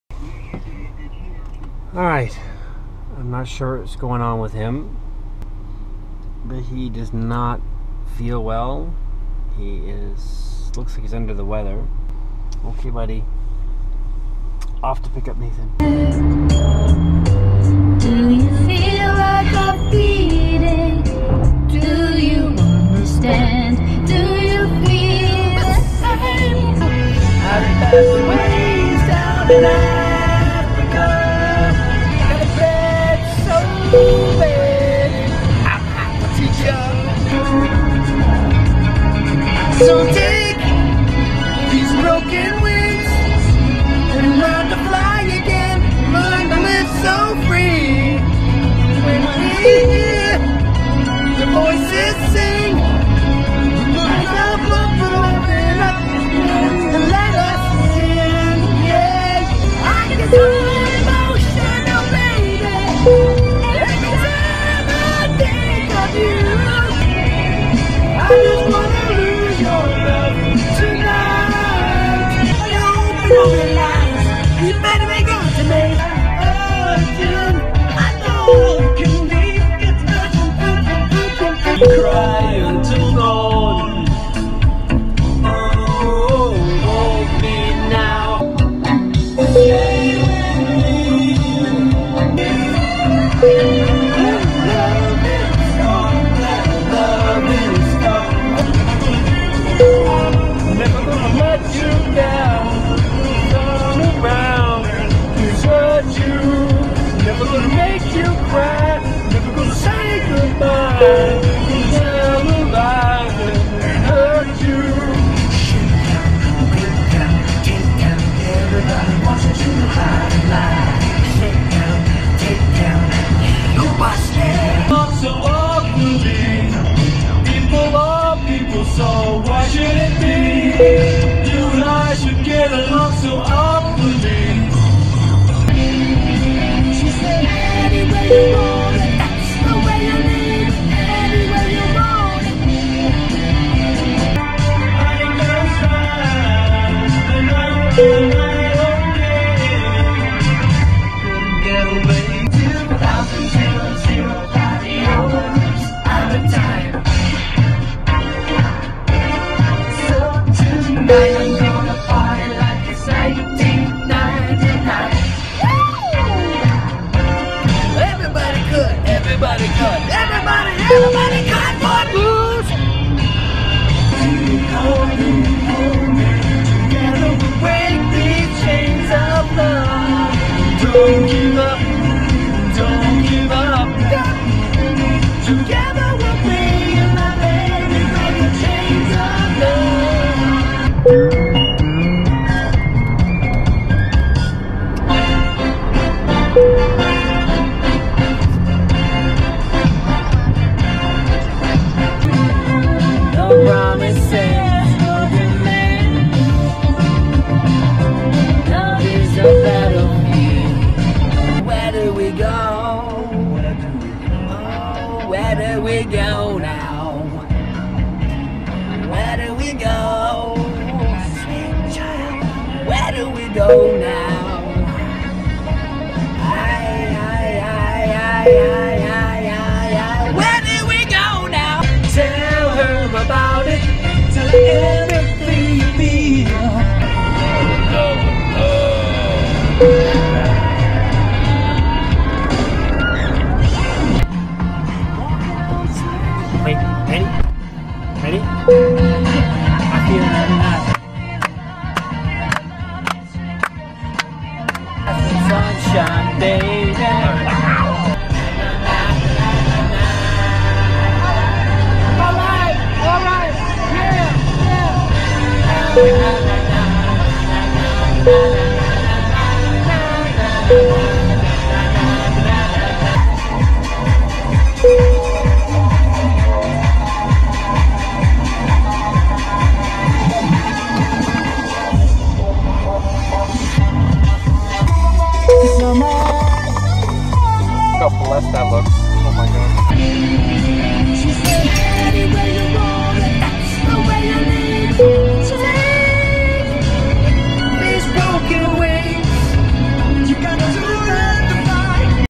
vlog 034 (5mn) - How to CARaoke
On my way to pick up the Boy for my weekend

All songs are owned by their respective artists. not me. I am just singing off of my Sirius